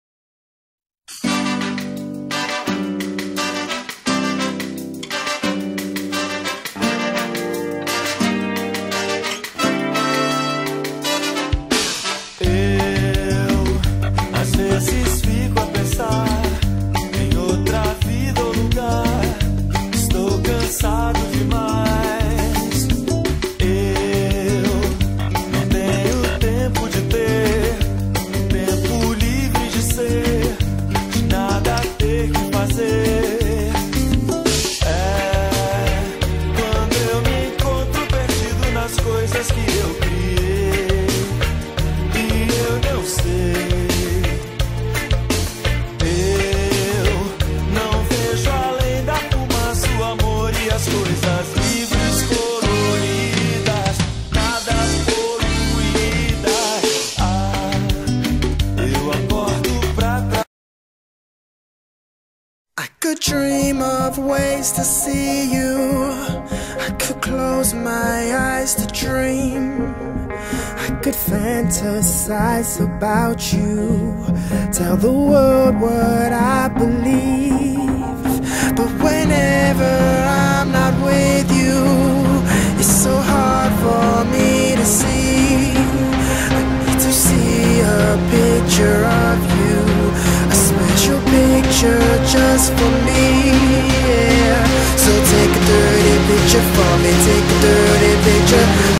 Trabalho de História- O dia do trabalhador- Stéphani Meneghel e Darah Silva
Projeto desenvolvido pelos alunos da E.E. "Profº Lourival Gomes Machado", sob a orientação do professor Marcos Aurélio.